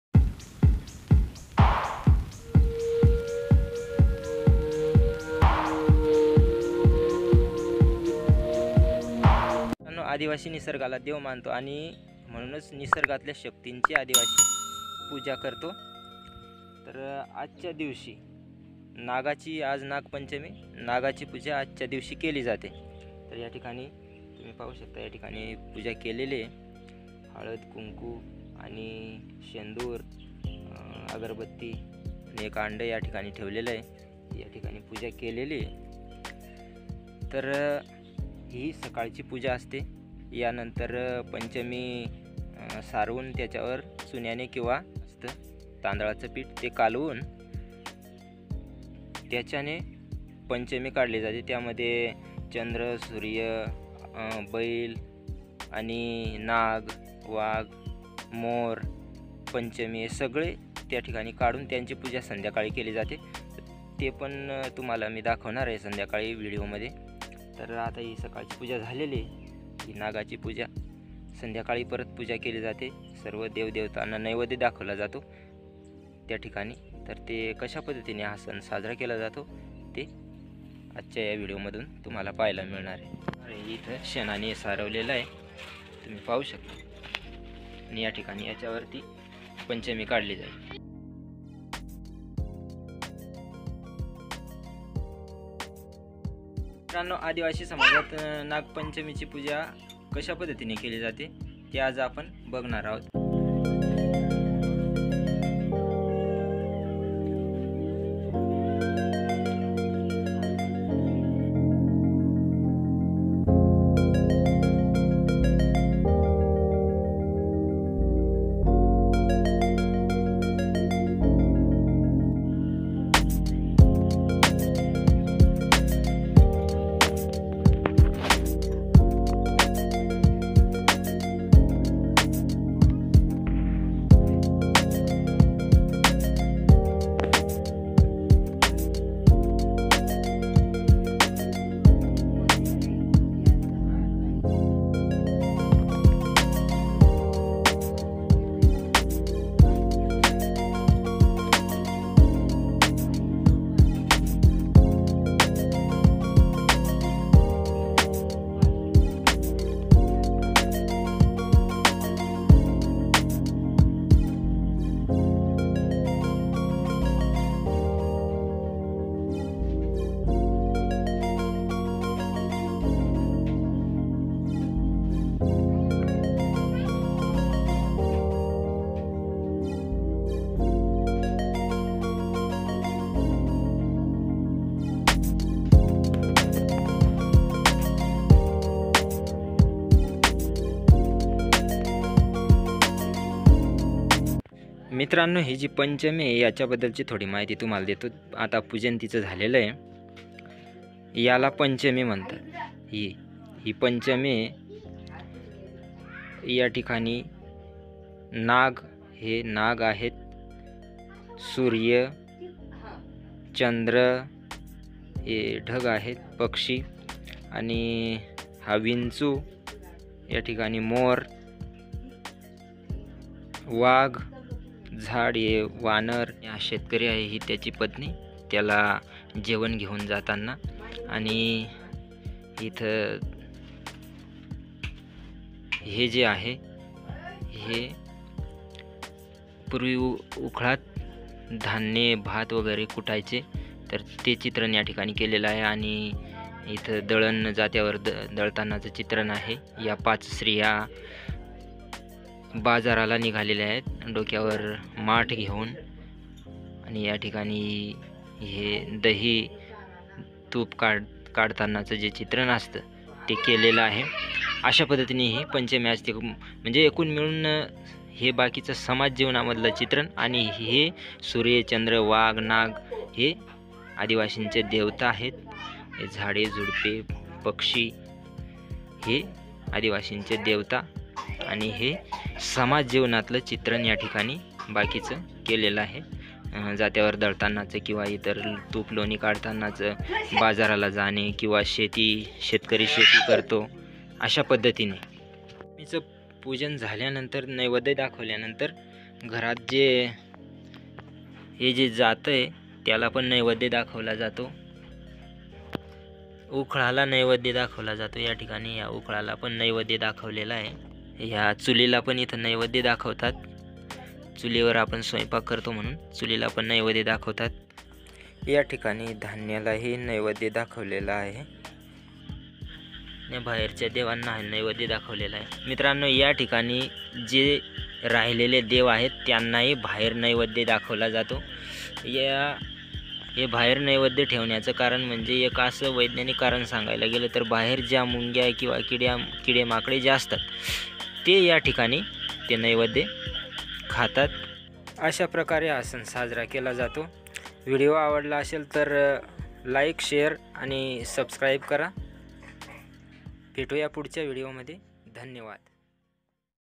नागपंचमी| आदिवासी सण| aadivasi nagpanchami aadivasi festival आदिवासी सण
आदिवासी नागपंचमी सण कसा साजरा करतात ती संपूर्ण माहिती या विडिओत सांगितली आहे विडिओ आवडल्यास नक्की लाईक शेयर आणि सबस्क्राईब करा
          धन्यवाद
आदिवासी सण